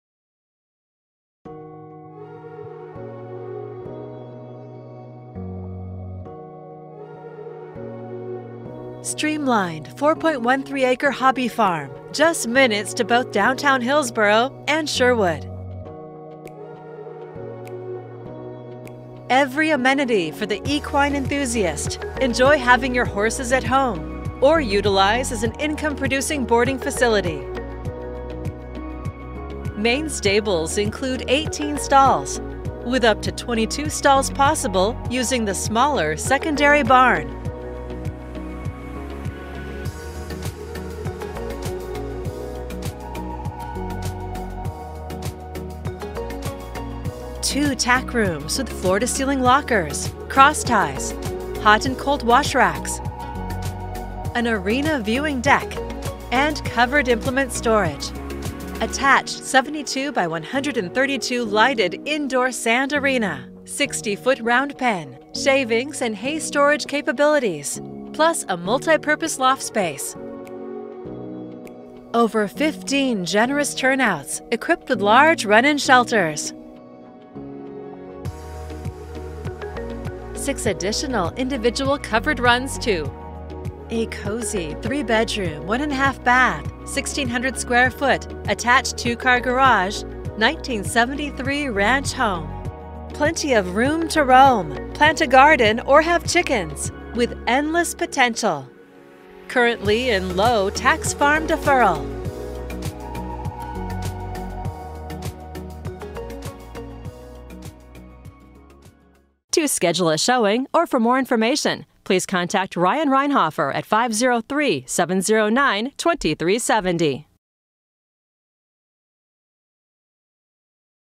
Hobby and Equestrian Farm on 4 Acres near Hillsboro ~ Video of 8730 SW River Rd. ~ Horse Properties
Streamlined 4.13 acre hobby farm, just minutes to both downtown Hillsboro and Sherwood. Every amenity for the equine enthusiast, enjoy having your horses at home or utilize as an income producing boarding facility. Contact Ryann Reinhofer with Thoroughbred Real Estate Group.